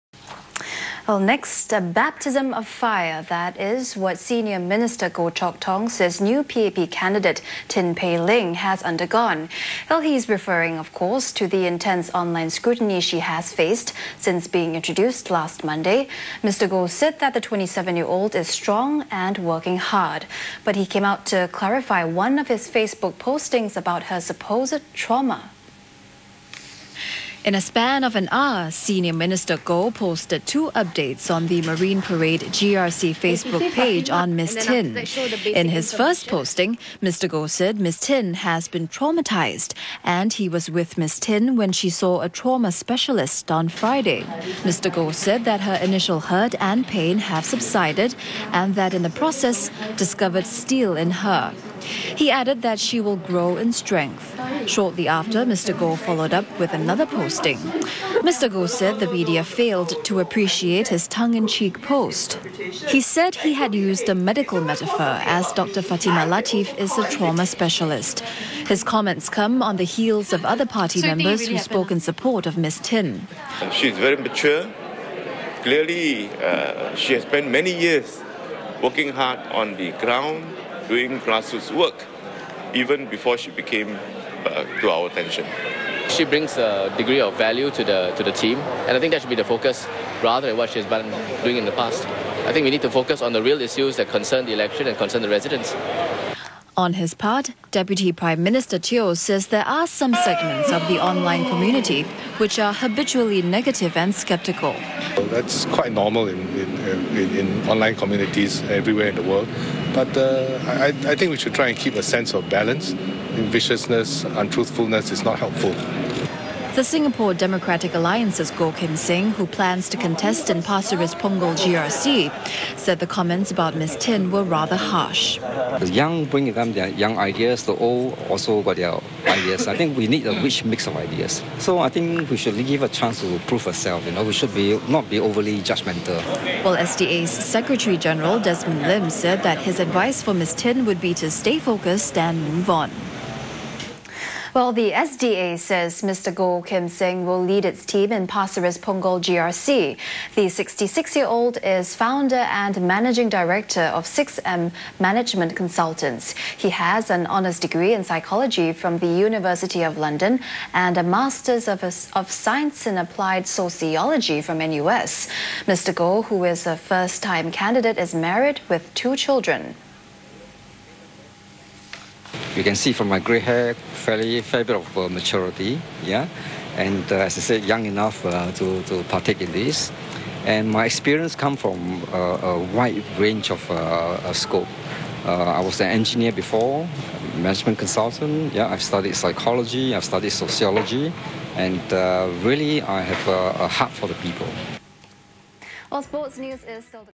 PAP youngest candidate Tin Pei Ling was traumatized - 02Apr2011
This time SM Goh Chok Tong, two DPMs Wong Kan Seng & Teo Chee Hean show support for Ms Tin~